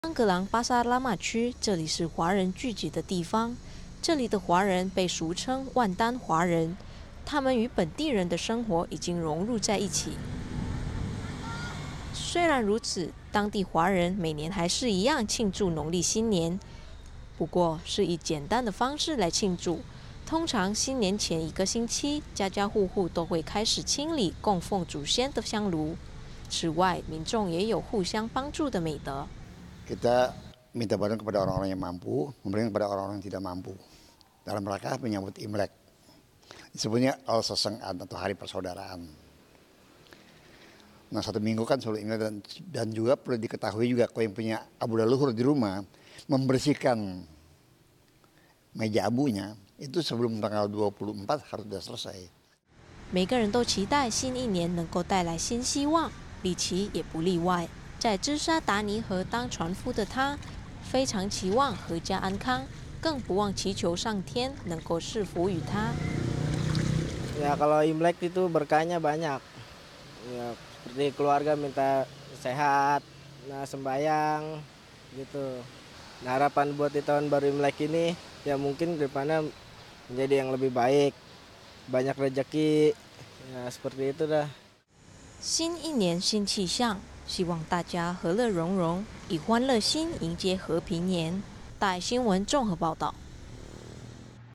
除舊布新 新年氣氛歡樂
越接近農曆新年，家家戶戶開始忙著大掃除，買一些新年必備品，歡樂迎接新年的來到。而在丹格朗的華人也會以簡單方式來慶祝，不過農曆新年的歡樂氣氛，還是少不了。

丹格朗Pasar Lama區，這裡是華人聚集的地方，這裡的華人被熟稱萬丹華人，他們與本地人的生活已經溶入一起。

雖然如此，當地華人每年還是一樣慶祝農曆新年，不過是以簡單的方式來慶祝，通常新年前一個星期，家家戶戶都會開始清理供奉祖先的香爐。此外民眾也有互助的美德。

寺廟管理員 魏金恩：「 我們呼籲有能力的人，幫助一些需要幫助的人，迎接農曆新年可以說是，團結的好節日，新年前要記得清理供奉祖先的香爐。」

每個人都期待新一年能夠帶來新希望，李啟也不例外。

在芝莎達尼河當船夫的他，非常期望闔家安康，更不忘祈求上天能夠賜福予他。

船夫 李啟 ：「 新年財源廣進，拜拜的時候也會祈求一家人健康，希望新年能帶來新氣象，多財多福。」

新一年新氣象，期望大家和樂融融，以歡樂心迎接和平年。

印尼綜合報導